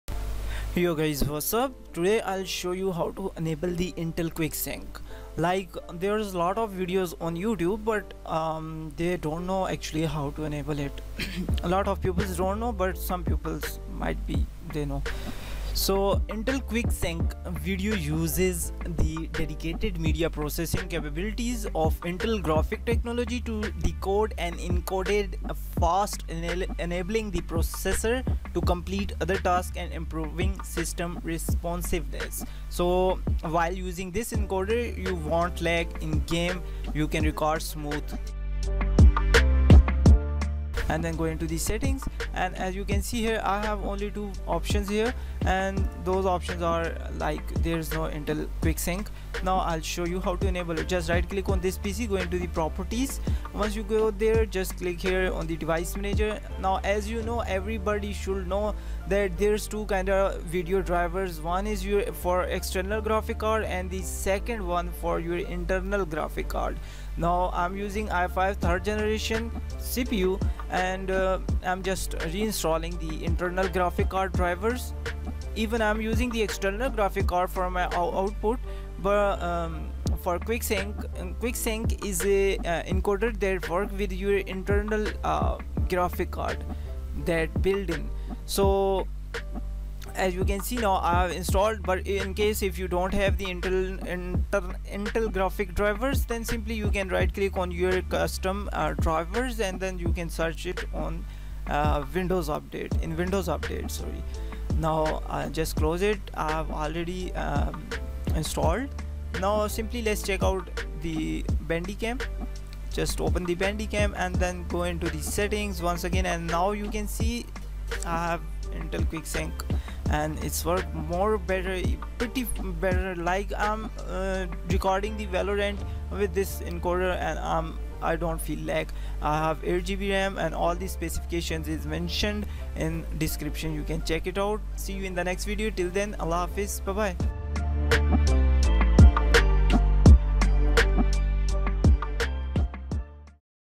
How to enable and use Intel® Quick Sync video codec (Bandicam) 🧬 Record Game Without Lag Now!
How to enable and use intel "Intel® Quick Sync video Codec" (Easy method). can we download the intel codec sync video coder and install it? yes, you can but not like tools or software.

✨ Simply you can download & install the intel internal graphic drivers and you will get intel quick sync and it's supportable only in i3 i5 i7 i9 intel series.

►Specs:
i5 3470 intel Processor
RAM: 8GB
GPU: gt610 1GB ddr3 64bit internal bandwidth
SSD: 128 ( Not effected by fps performance )
HHD: 250GB